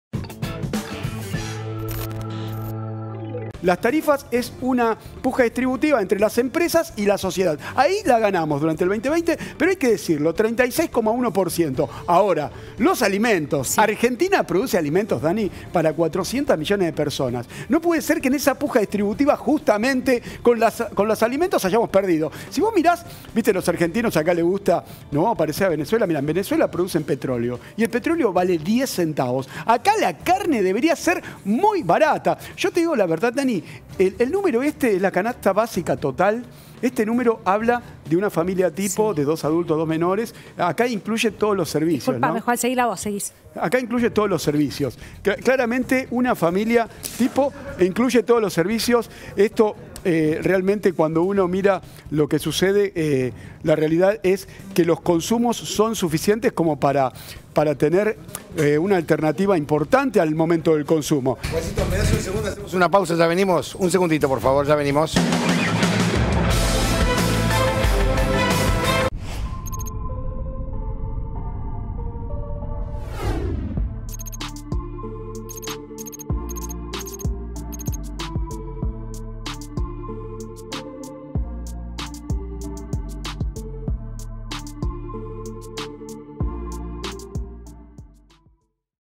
Una periodista se desmayó en medio de un informe en C5N
Daniela Ballester estaba junto a su compañero de piso Juan Enrique presentando un informe de economía cuando se escuchó: “Seguila vos”.

ID6